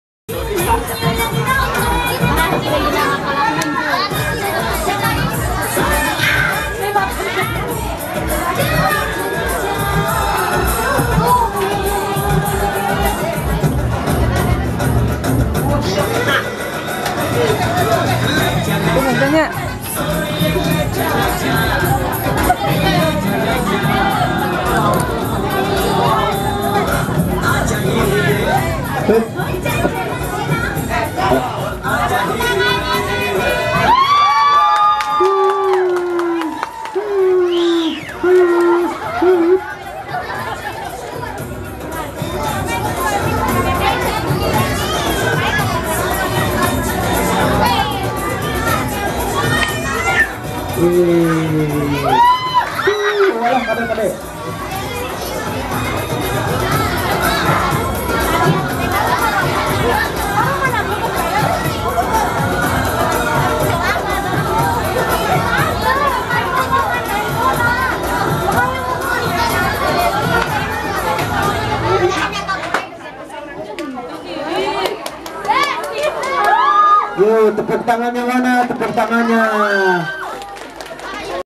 (Koreografi) Mutiara DKK
Dalam Acara
Perpisahan & Tabligh Akbar
Peserta PLKJ PPI 113 Izhaarul Haq
Di
Desa Pasirlangu, Pakenjeng, Garut